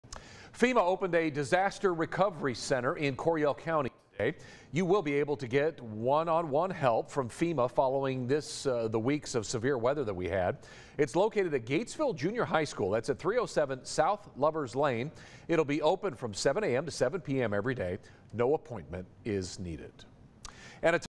FEMA opens Disaster Recovery Center in Coryell County for those affected by severe storms
The center will be open at Gatesville Junior High from 7 a.m. to 7 p.m. daily.